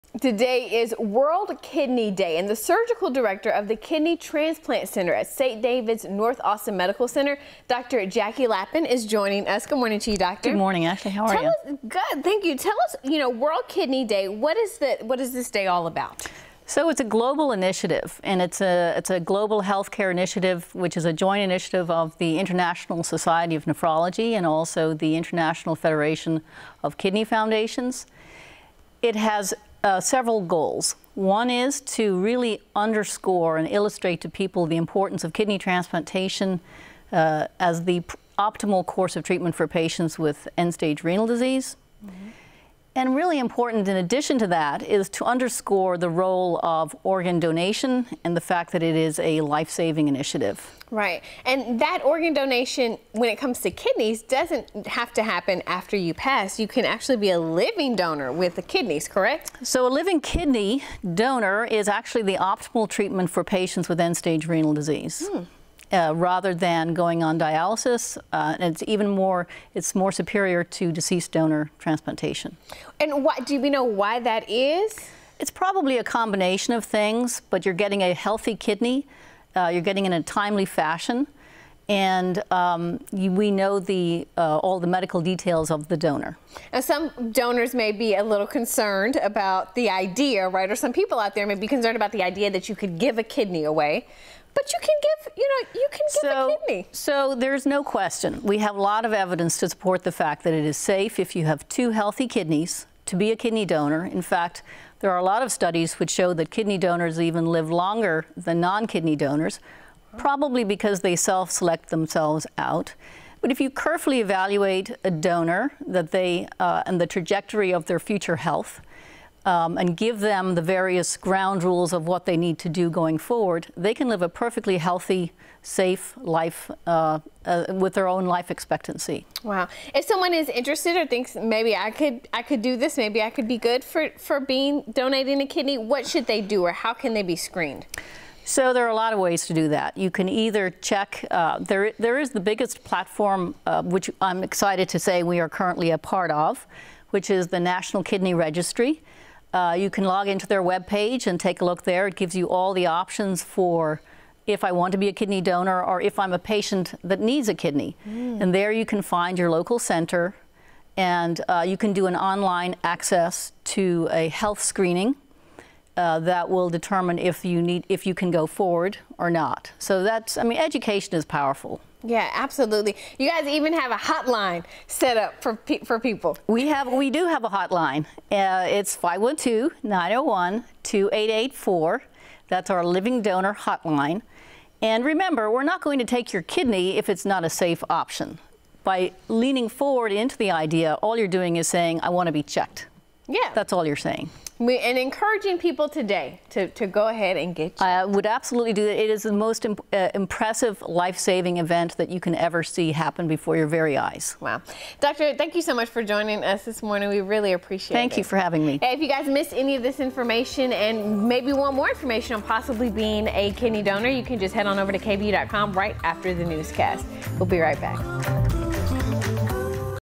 Discussing kidney health on World Kidney Day | KVUE
Dr. Jacquie Lappin with St. David’s North Austin Medical Center joined KVUE to talk about it.